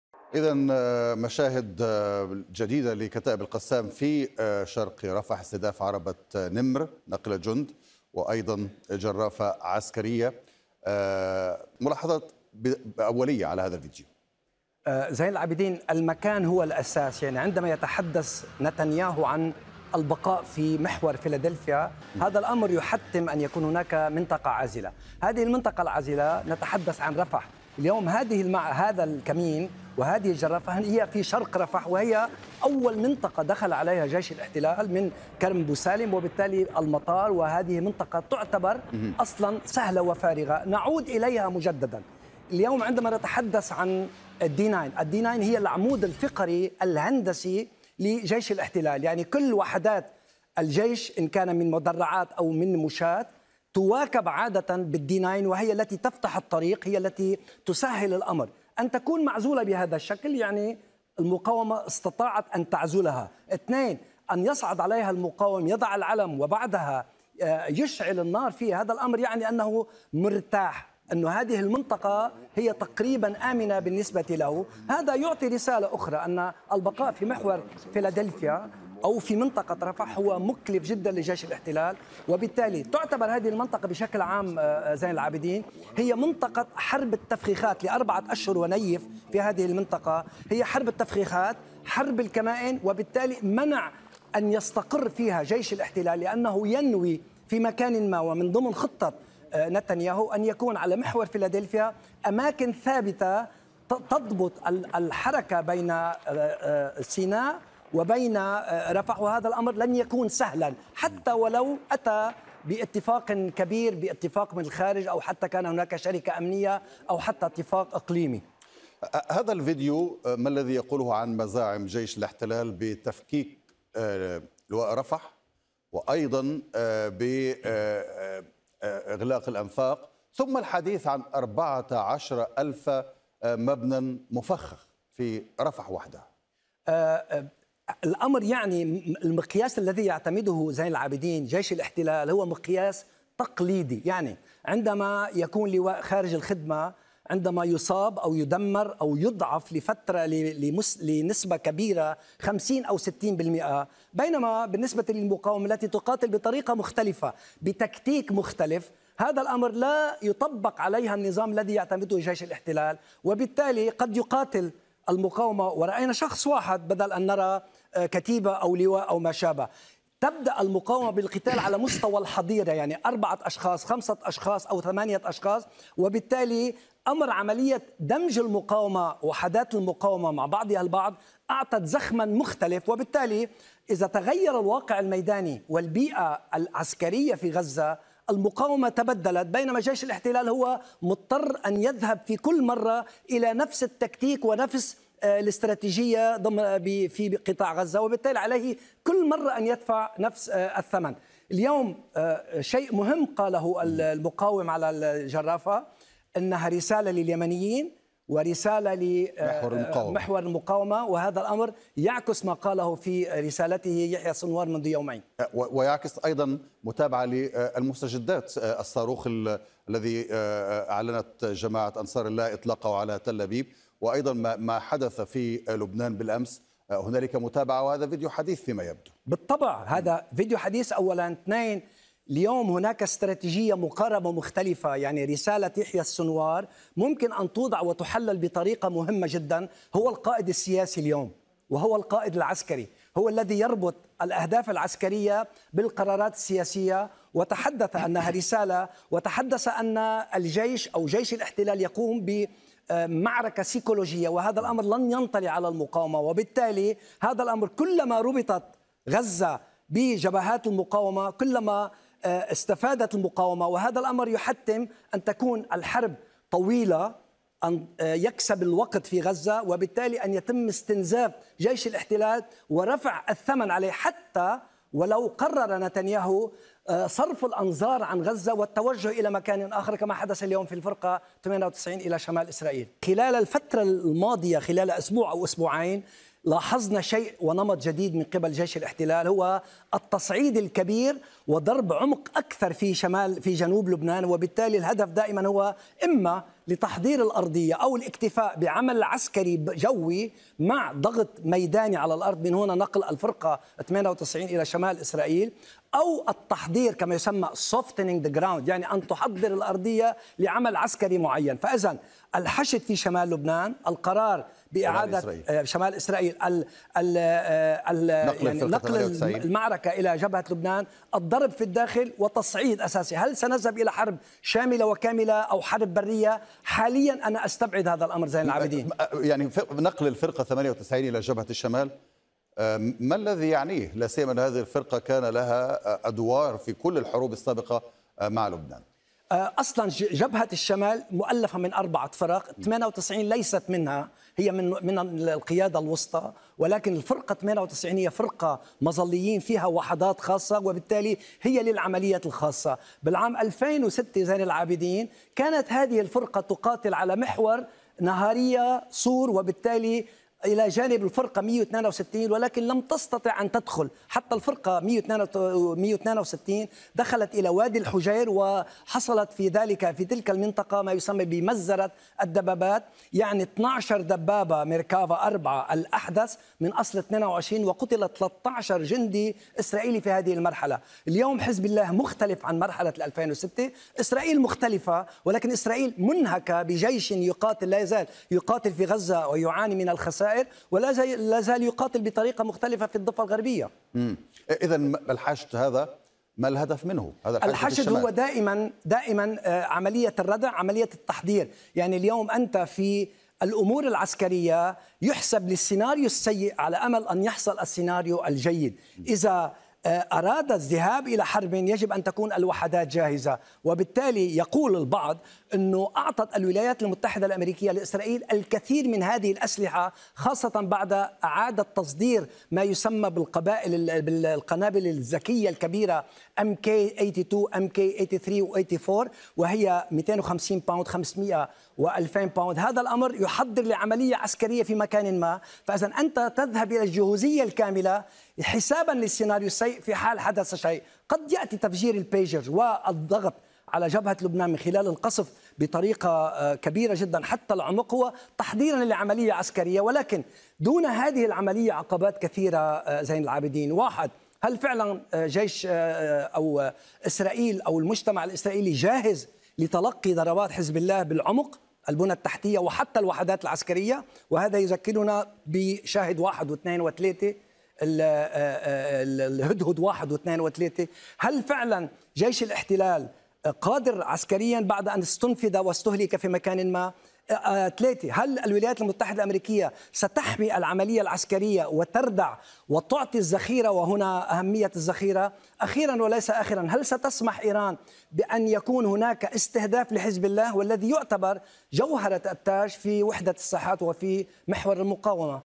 القسام تـدمر 3 ناقلات جند "نمر" ب"عبـوات" و"إلياسين" و"تاندوم"..ومقاوم من القسام يوجه رسالة لإسرائيل
فقرة تحليلية لآخر مستجدات المواجهات بين عناصر المقاومة و قوات الاحتـلال.. التحليل مع الخبير و المحلل العسكري و الاستراتيجي.